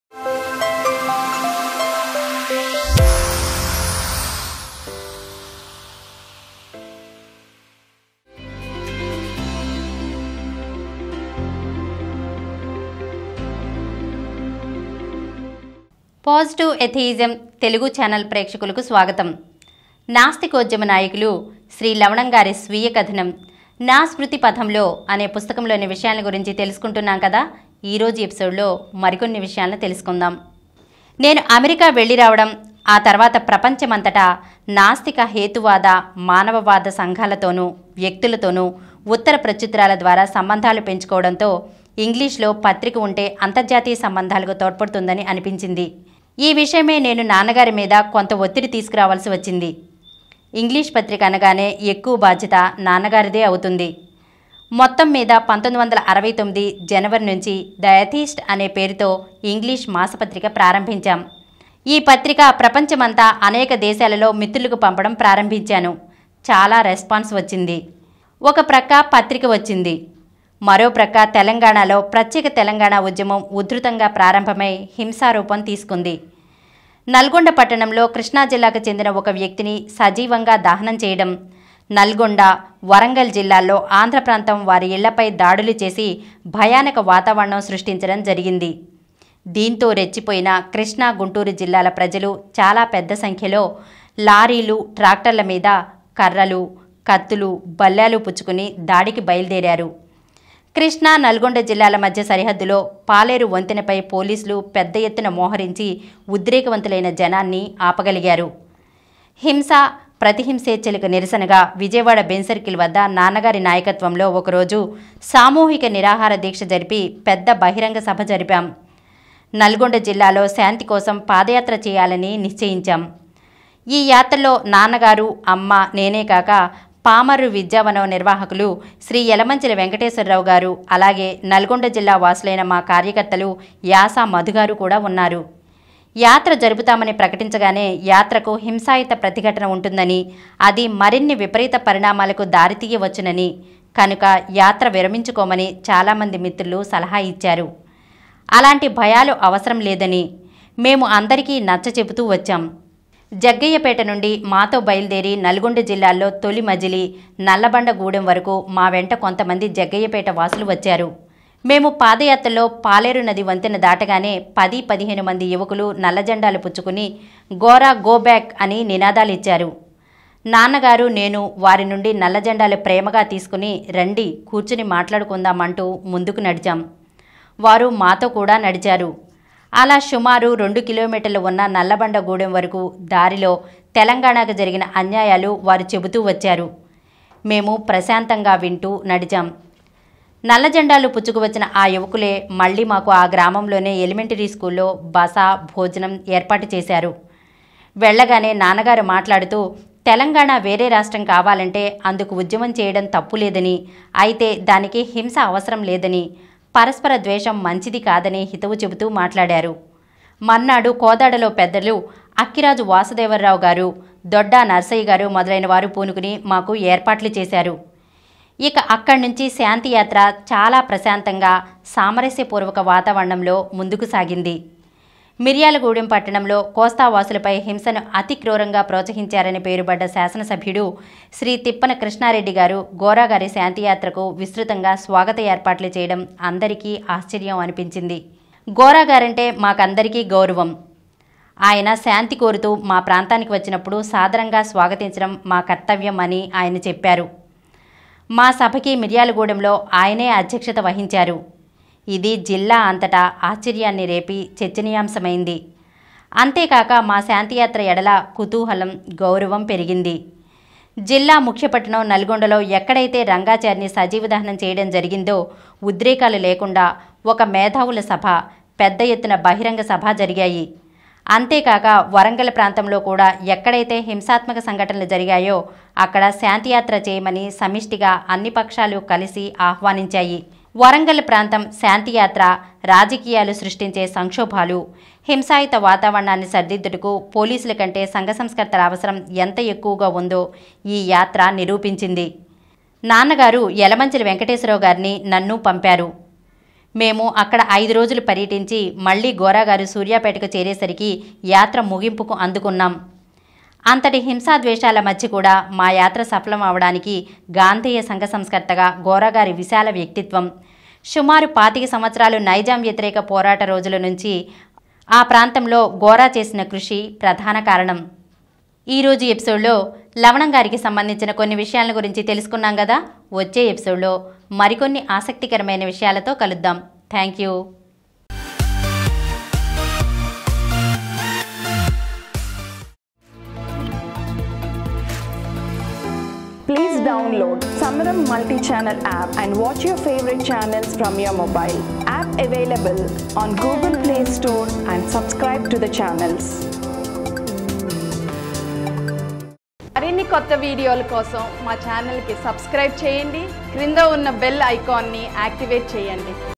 దీనితో రెచ్చిపోయి కర్రలు, కత్తులు, బల్లేలు పుచ్చుకుని దాడికి బయలుదేరారు(ఎపిసోడ్-66)in నా స్మృతిపథంలో
NAA SMRUTI PATHAM LO (Episode- 66) by Lavanam. Read by Lakshmi Prameela in Atheist Centre Channel.